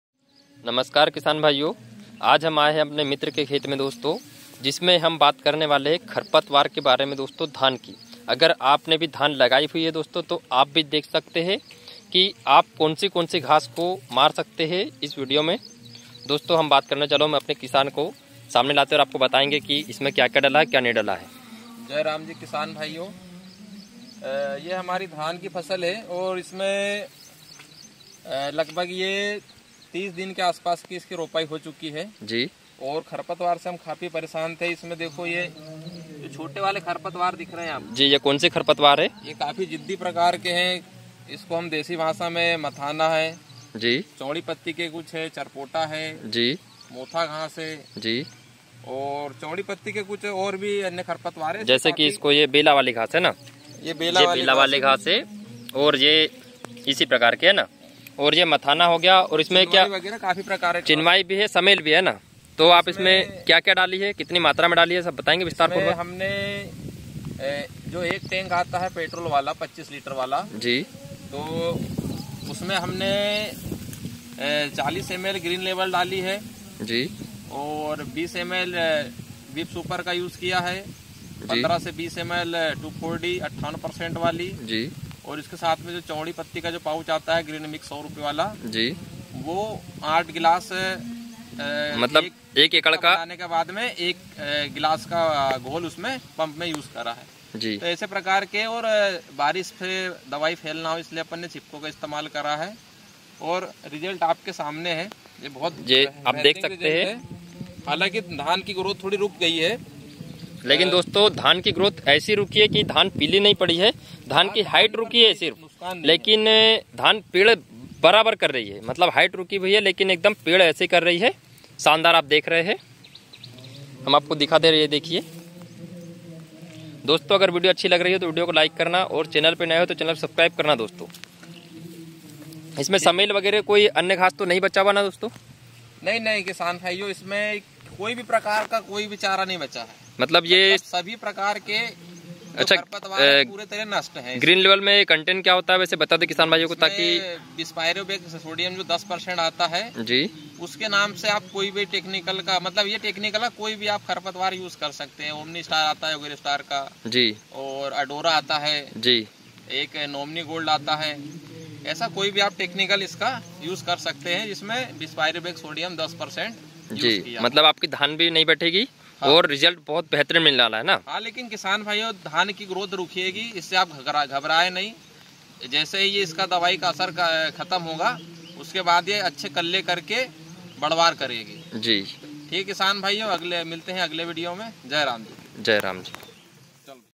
dhan me dale ye kharpatvar davai ek hi spre me chara saf kisan #dhan
dhan me dale ye davai or chara ka kre puri trah ka khatma dhan ki groth bhi nhi ruke chara ko. kad se khutum karta he